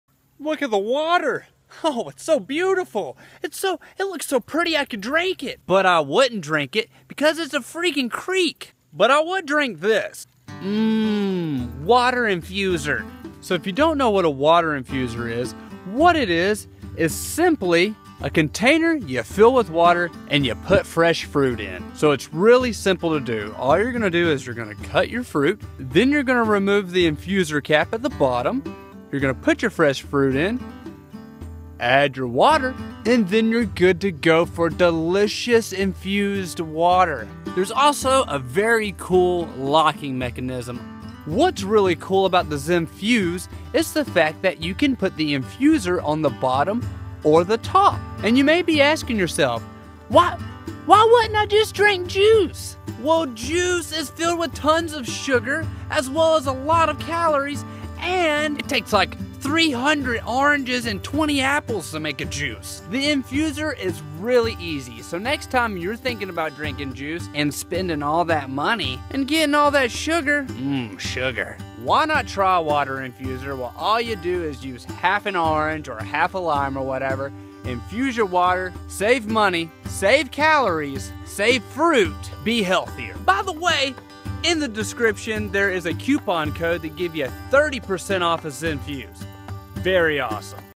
Water Infusers: Zenfuze - Health Tips
Water Infusers are a great way to reduce sugars & calories, while tasting amazing!

SOCIAL ME!

80/20 Splits, One on one consultants, Tips, Editors, & More.
Over 30 ways to monetize.